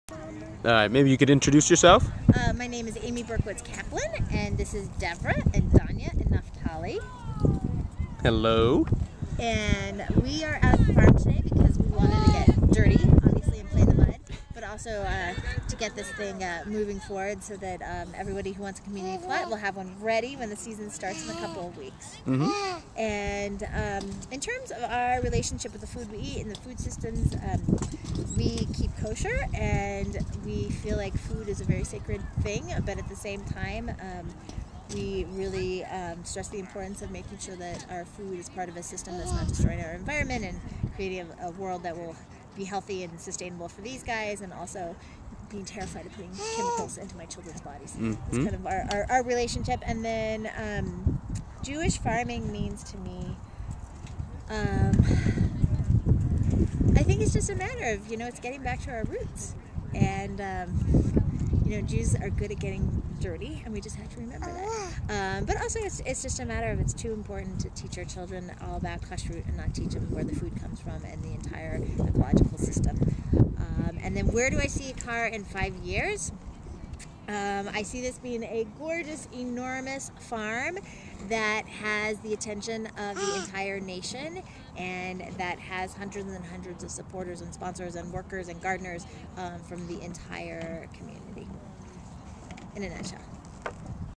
Ekar Farmers - Amy Berkowitz Caplan
Amy and family take a break during the April 25th Day in the Dirt.